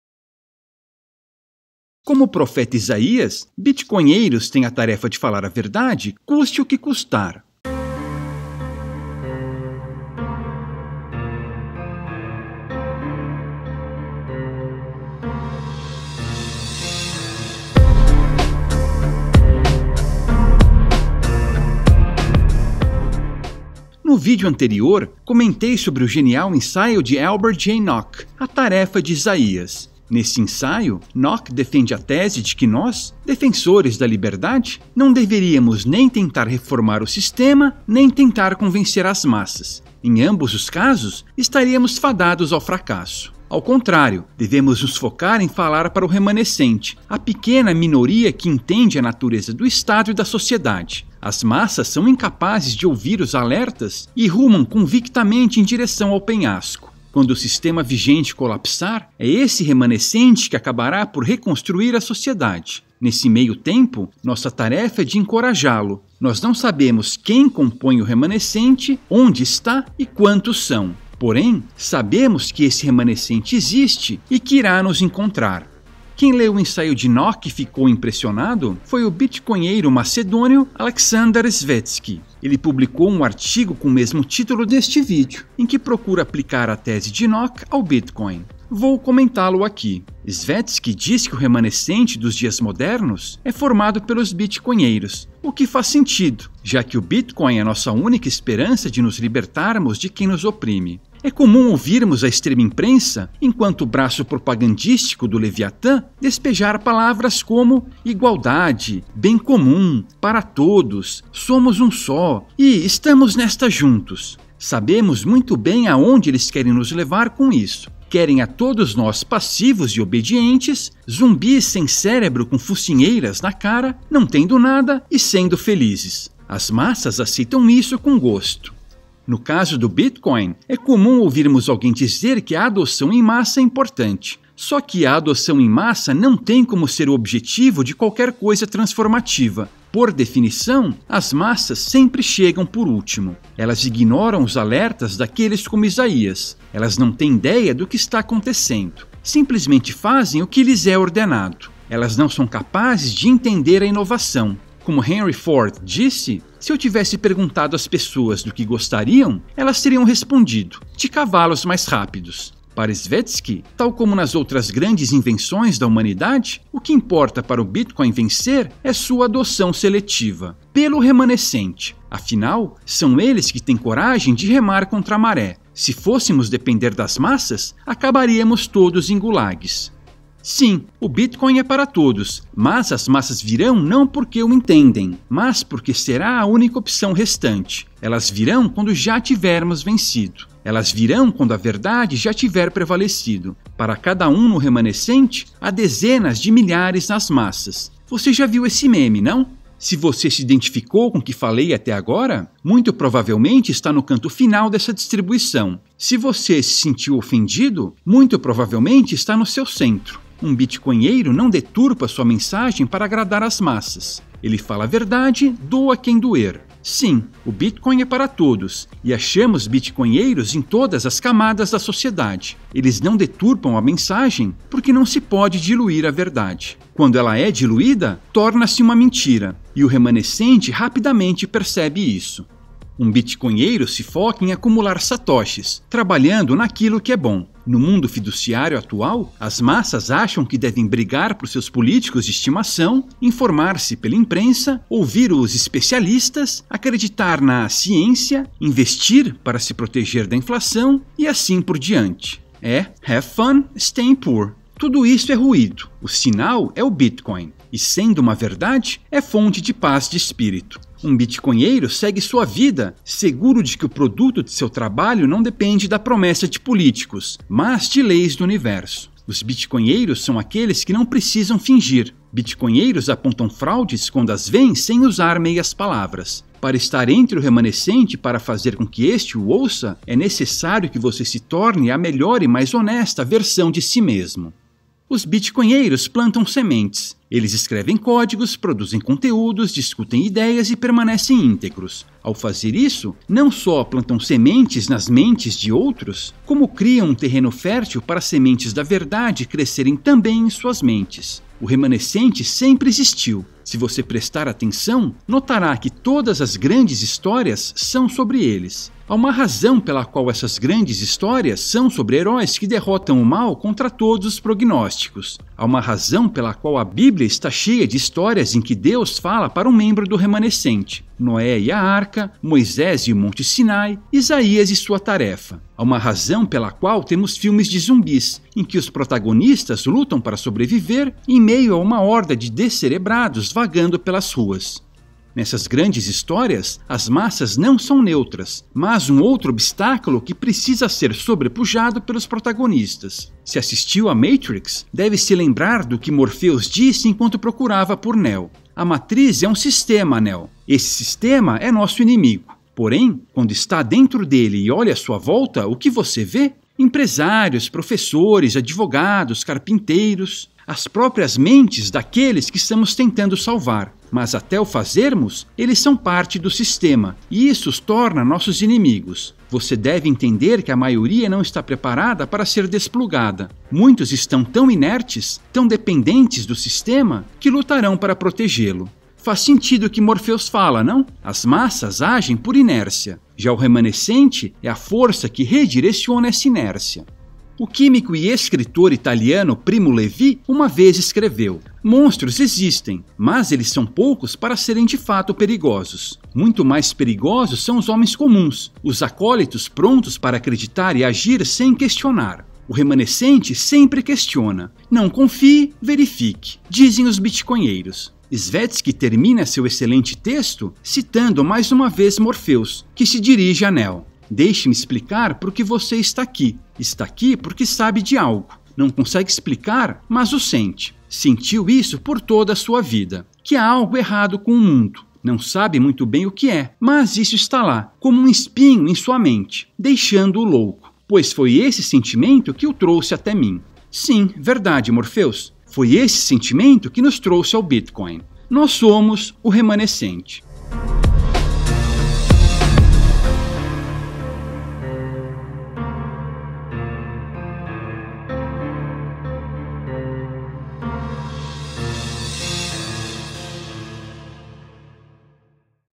Bitcoinheiros São O Remanescente
A tese de Albert Jay Nock, de que devemos falar para o remanescente e não para as massas, pode ser aplicada ao bitcoin. Os bitcoinheiros, por se manterem íntegros e falaram a verdade custe o que custar são o remanescente e falam para este. O bitcoin é, sim, para todos, mas as massas o adotarão por inércia, quando já não houver outra opção, quando a luta já tiver sido vencida. Fontes A propósito, se quiser e puder apoiar o projeto com uns satoshinhos, ficarei grato: tallyco.in/marcobatalha/